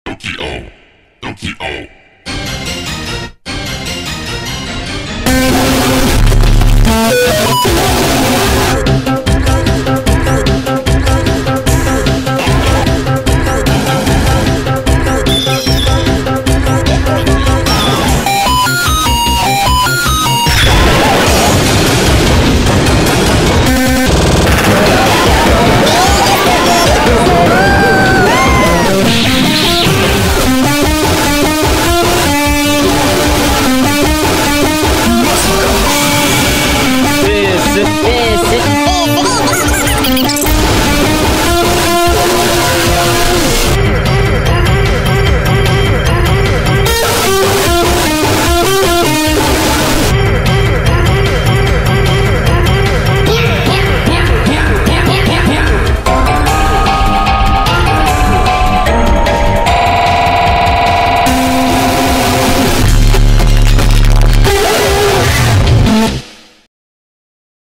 復活の「H」（音割れホラーマンVS神素材オールスター）
ニコニコと共に蘇った音割れホラーマンがニコニコ支配を目論む！？
果たしてキレるプルート一行は彼の侵略を阻止できるのか？